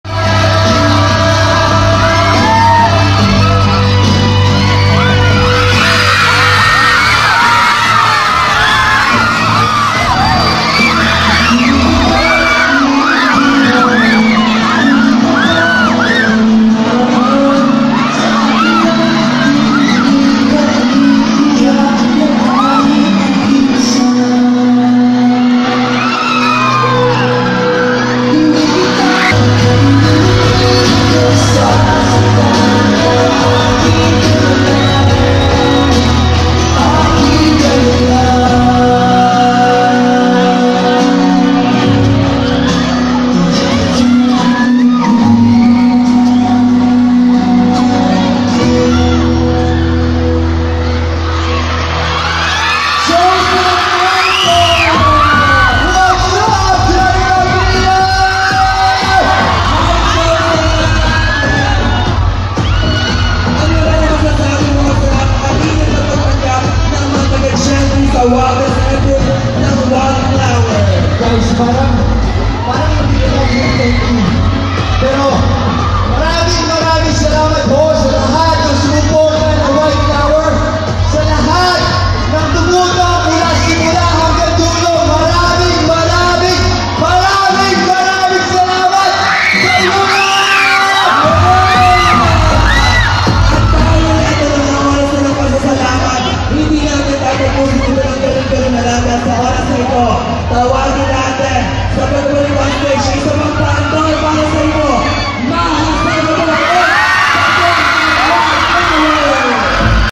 Joseph Marco / Diego Torillio in WildFlower Pasasalamat Mall Show
Live Viewing and Mall Show of the Wild Flower Cast 🔥